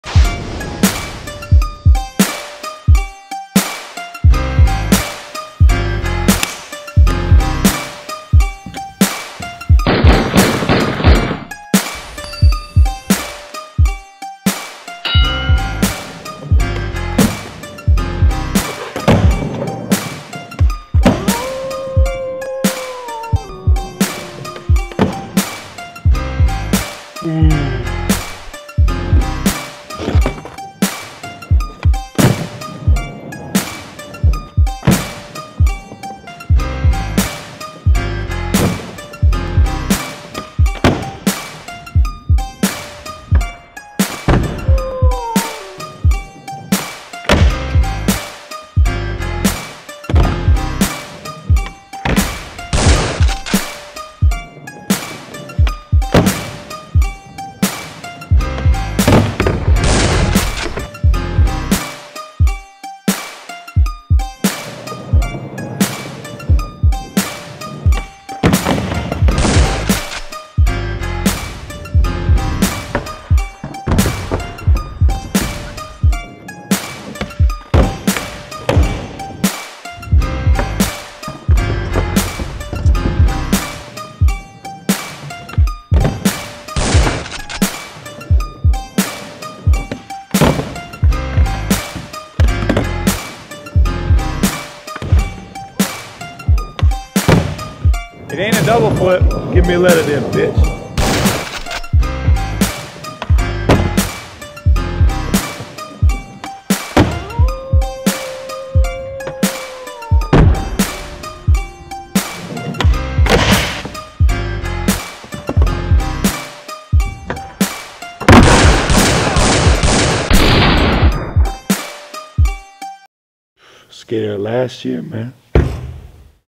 S.K.A.T.E-Chris Cole VS Larry Perkins
Chris Cole and Larry Perkins Battle it out in a game of S.K.A.T.E